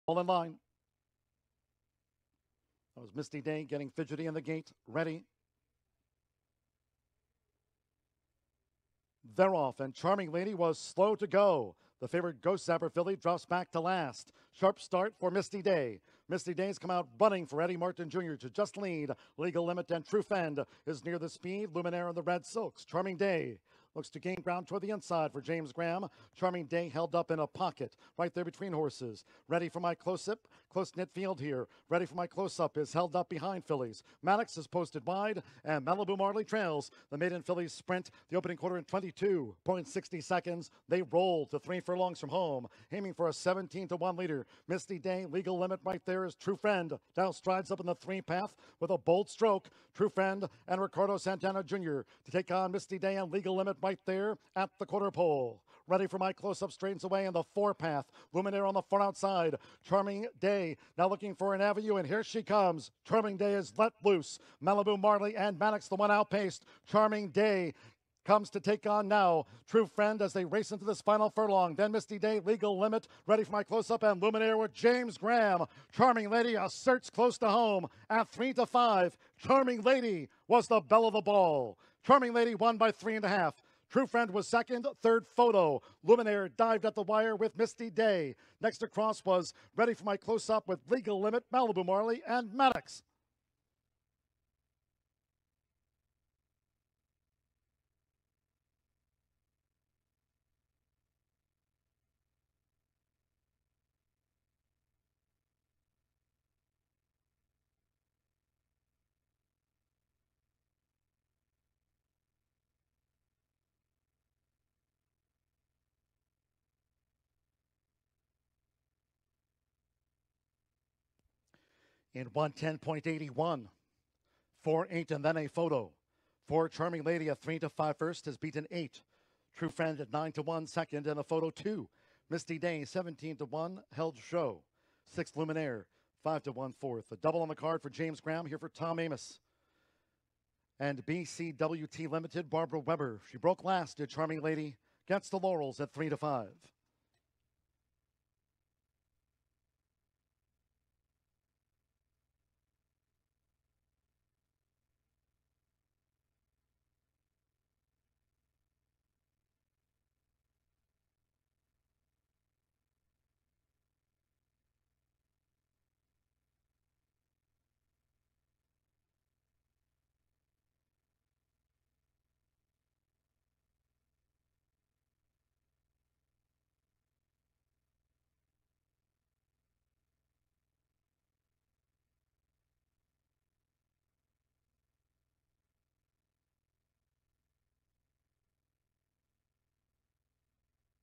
Luminaire Career Debut
Luminaire makes her career debut for Steve Margolis on January 18, 2020 at the Fair Grounds. She finished a solid fourth for jockey Chantal Sutherland.